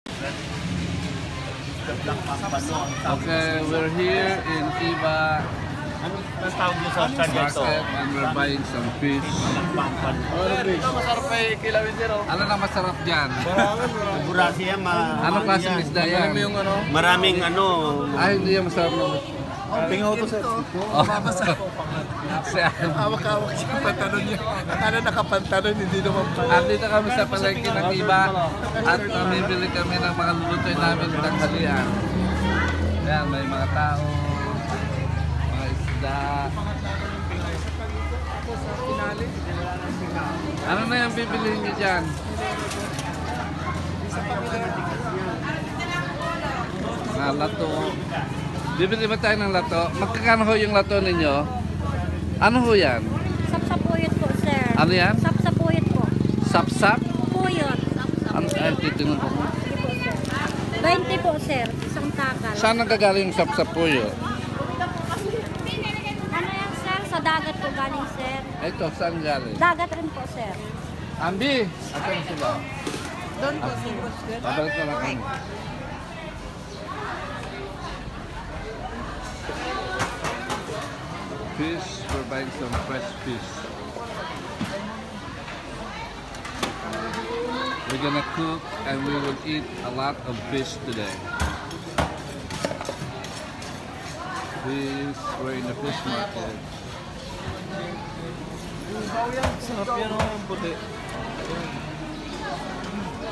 Buying fresh fish at the Wet Market
This video will showcase the freshness of living in a province near the fishing Village.